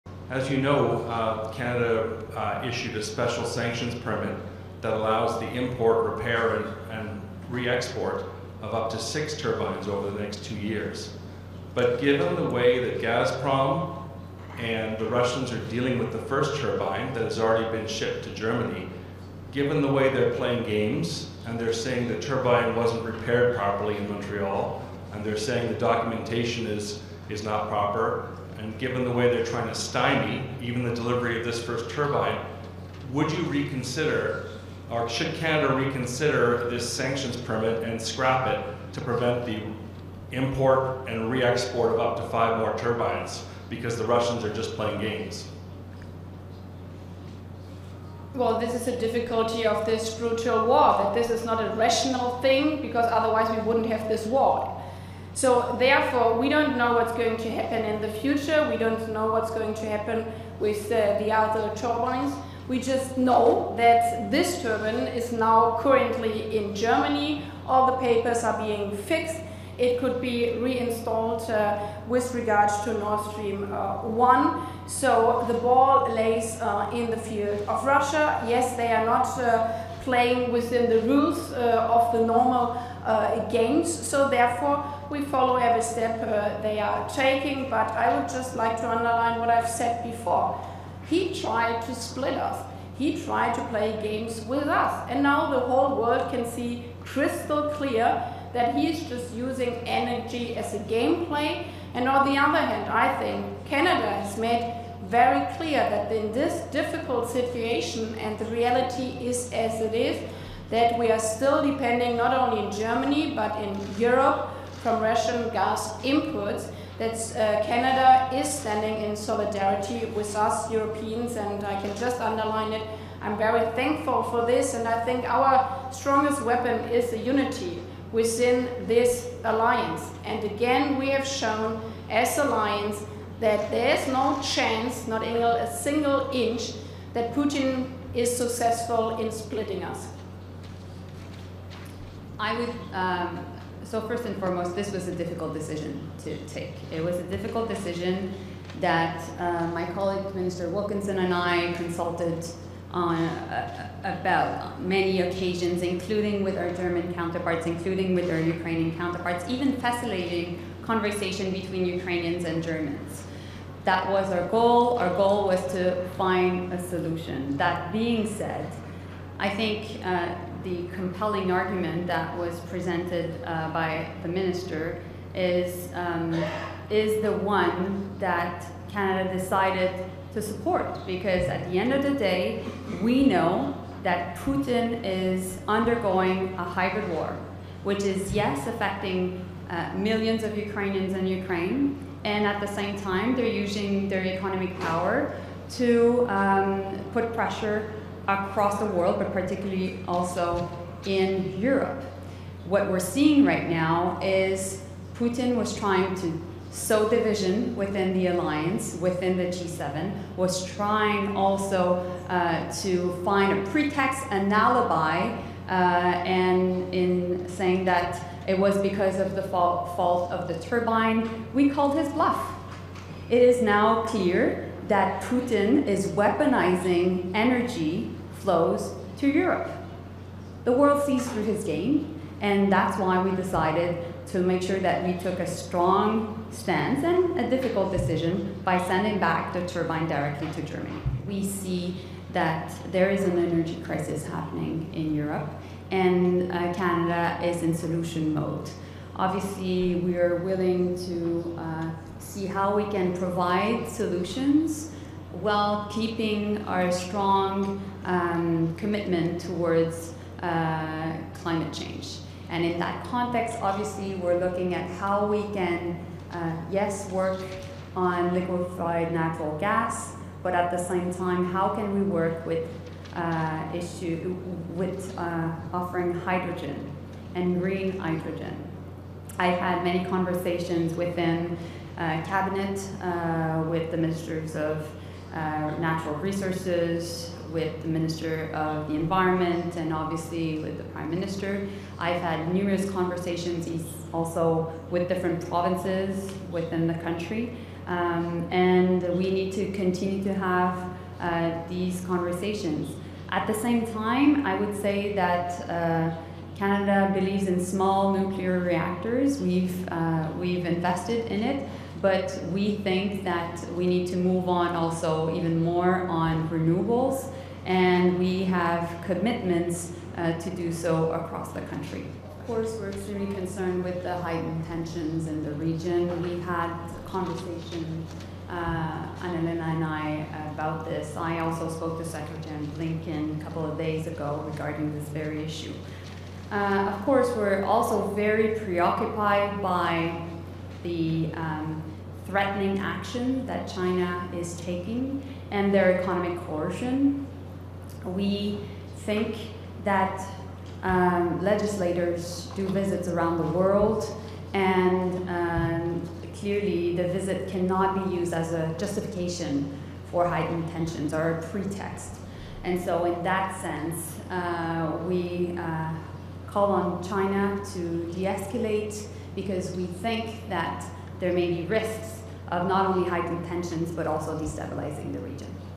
Canada called Putin’s bluff with turbine return for Russian pipeline: Joly
Canada’s Foreign Affairs Minister Mélanie Joly says that Russian President Vladimir Putin is playing games with G7 countries over energy exports to Europe.

“We called his bluff. It is now clear that Putin is weaponizing energy flows to Europe. The world sees through his game and that’s why we decided to make sure we took a strong stance and a difficult decision by sending back the turbine directly to Germany,” Joly said during a news conference alongside her German counterpart, Annalena Baerbock, in Montreal on Wednesday.

Canada issued a special sanctions permit that allowed the import, repair and re-export of up to six Russian turbines in a bid to help Germany as it struggles with energy supply.

Russia has claimed that the turbine Canada returned to Germany wasn’t properly repaired and lacks the proper paperwork.